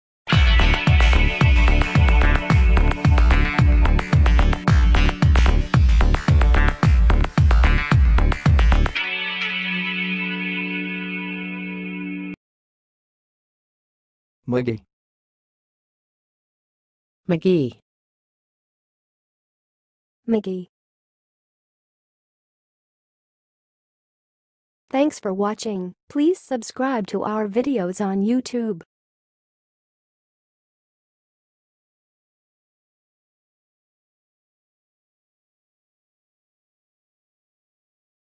How to say "mcgee"! (High Quality Voices)
Watch in this video how to say and pronounce "mcgee"!